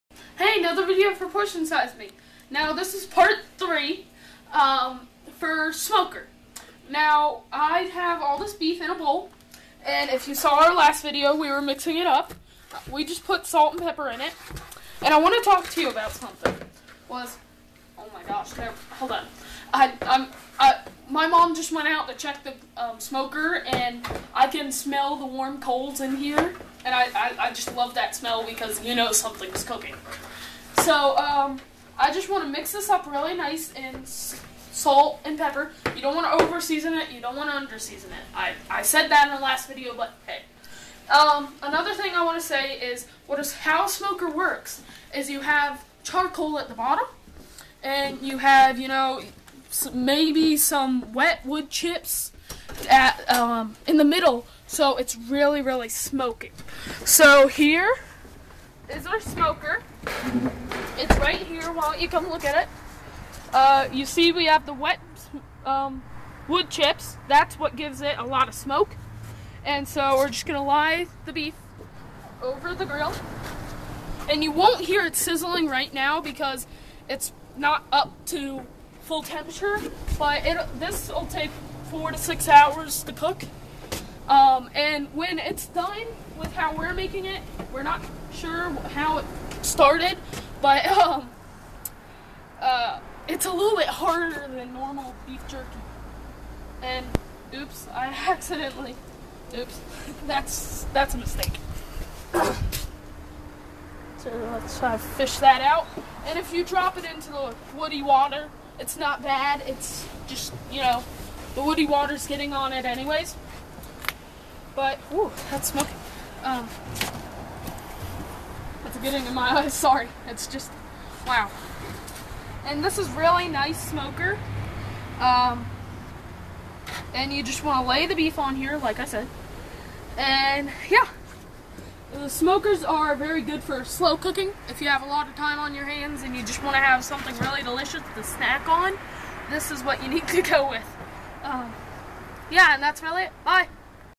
Portion Size Me: Smoked Beef Jerky #3
Video #3 of smoked beef jerky.  Go over to facebook and see the still photos of the completed product.

Remember the Portion Size Me goals: slow down and think about what you are eating:  read ingredients and make sure you understand what is in the items you choose:  Select lower processed food and look for items sore sale closer to home.